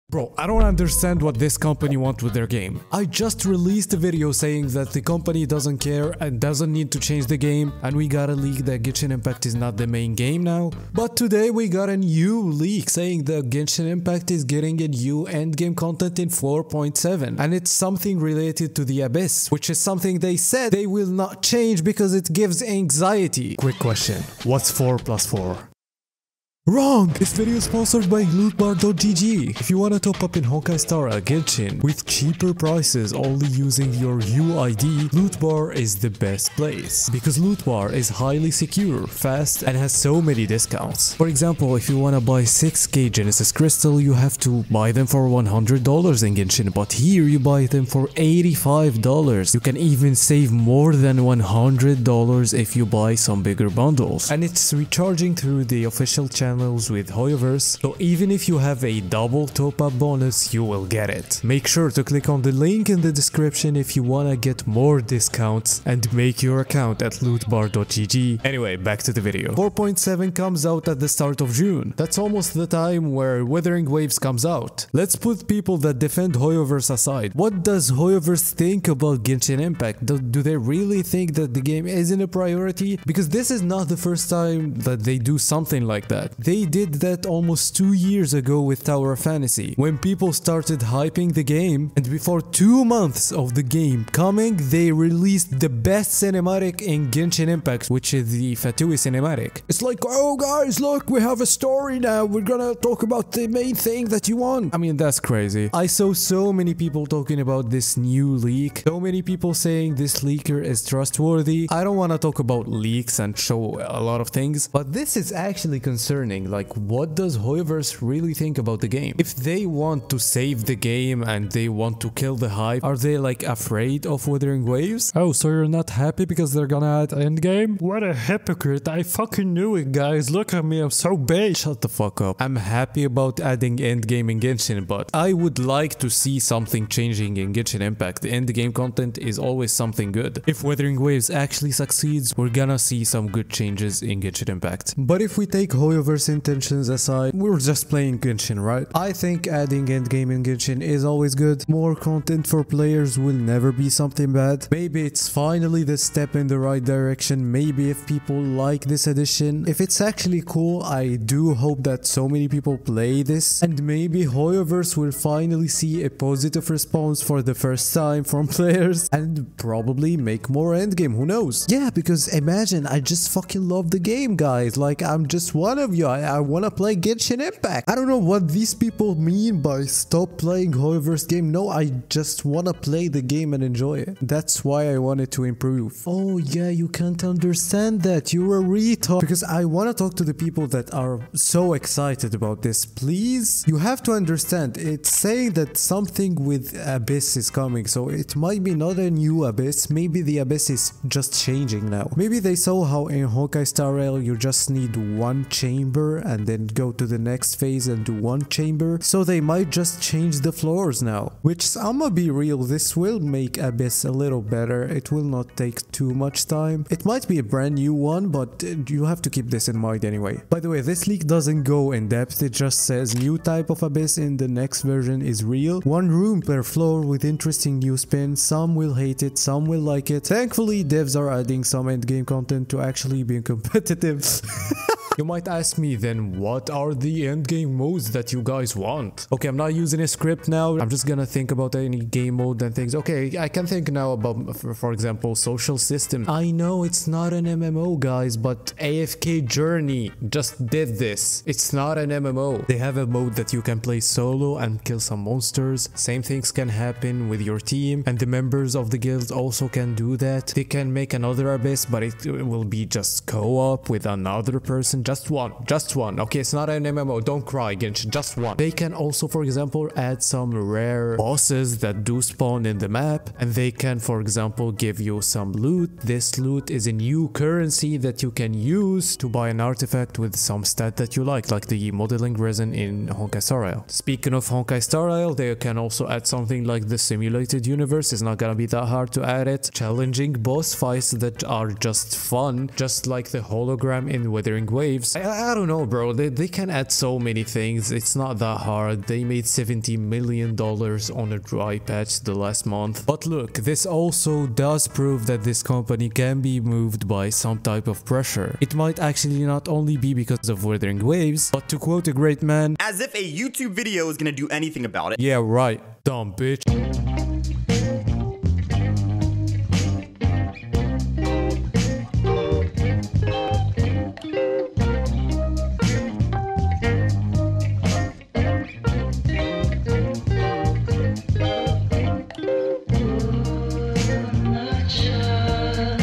Genshin Accidentally Leaks NEW End Game And it's Concerning !!!
00:00 intro
00:22 Sponsor
01:09 We have a problem
03:54 How to improve the game
05:02 moral of the story (important)